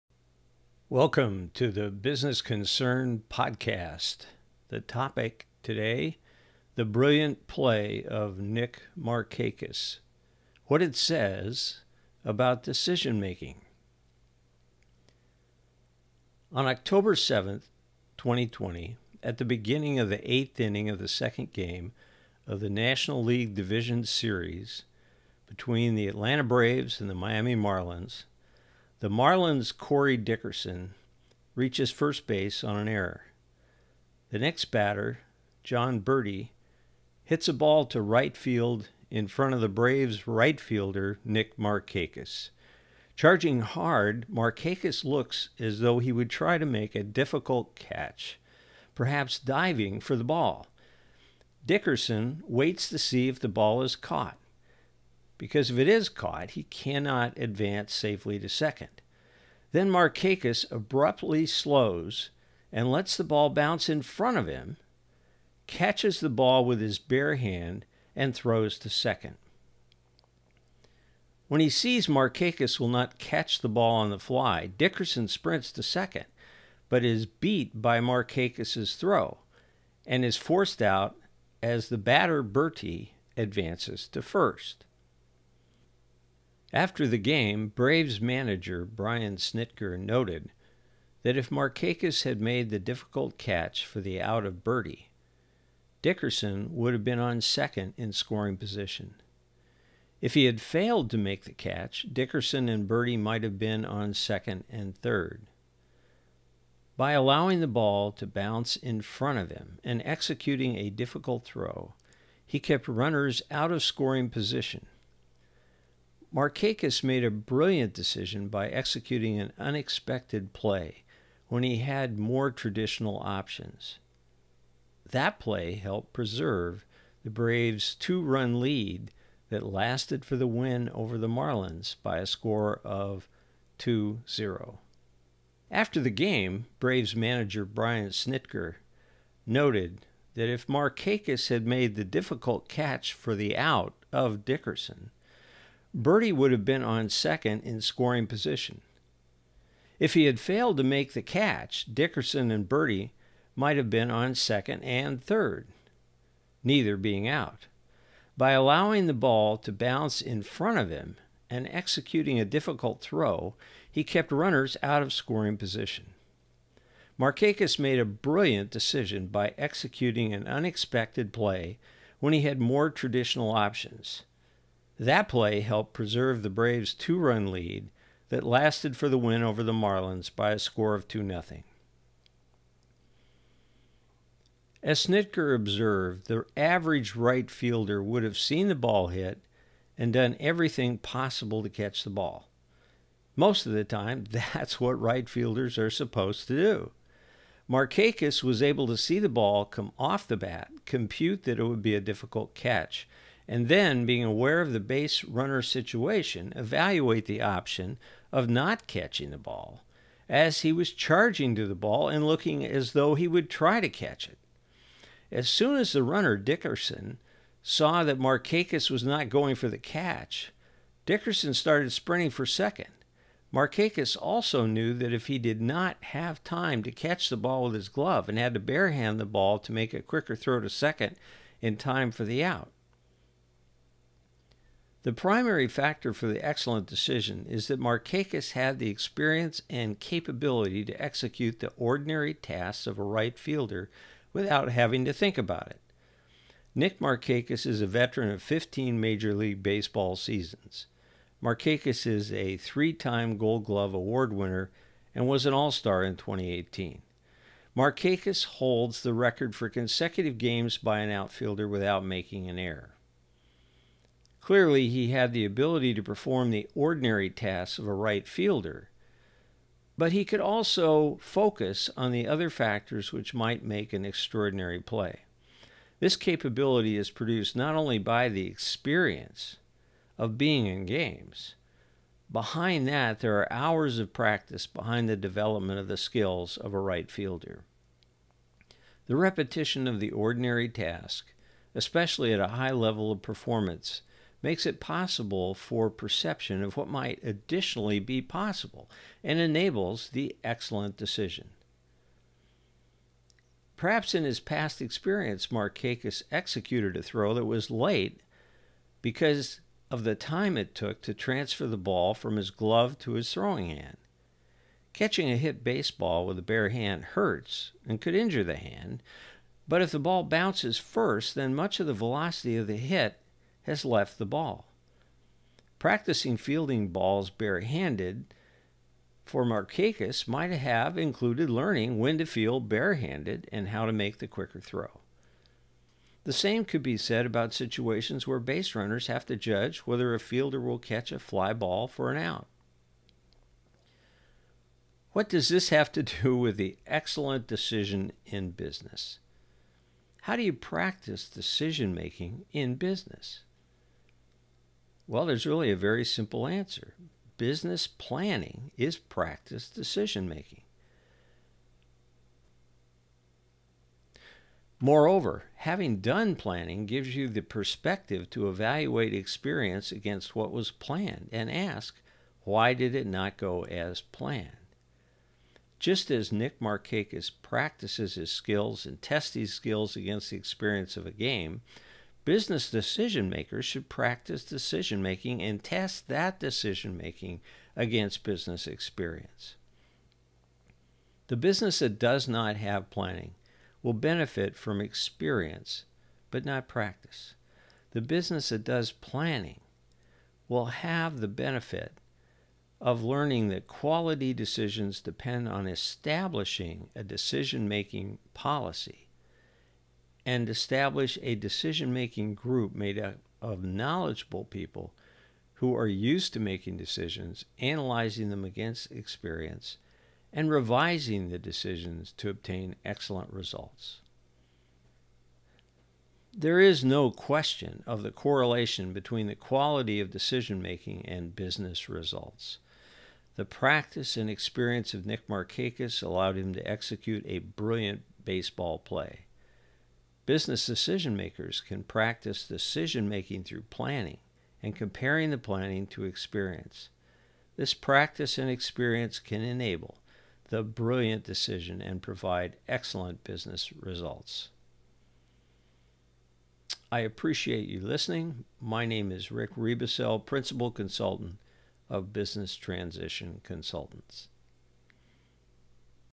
The Brilliant Play of Nick Markakis – What It Says About Decision-Making
On October 7, 2020, at the beginning of the eighth inning of the second game of the National League Division Series between the Atlanta Braves and the Miami Marlins, the Marlins’ Corey Dickerson reaches first base on an error. The next batter, Jon Berti, hits a ball to right field in front of the Braves’ right fielder, Nick Markakis. Charging hard, Markakis looks as though he would try to make a difficult catch, perhaps diving for the ball. Dickerson waits to see if the ball is caught because if it is caught he cannot advance safely to second. Then Markakis abruptly slows and lets the ball bounce in front of him, catches the ball with his bare hand, and throws to second. When he sees Markakis will not catch the ball on the fly, Dickerson sprints to second but is beaten by Markakis’ throw and is forced out as the batter, Berti, advances to first.

After the game, Braves manager, Brian Snitker, noted that if Markakis had made the difficult catch for the out of Berti, Dickerson would have been on second in scoring position. If he had failed to make the catch, Dickerson and Berti might have been on second and third. By allowing the ball to bounce in front of him and executing a difficult throw, he kept runners out of scoring position. Markakis made a brilliant decision by executing an unexpected play when he had more traditional options – that play helped preserve the Braves two-run lead that lasted for the win over the Marlins by a score of 2-0. What does this have to do with the excellent decision in business? How do you practice decision-making in business? The answer is that business planning is practice decision-making.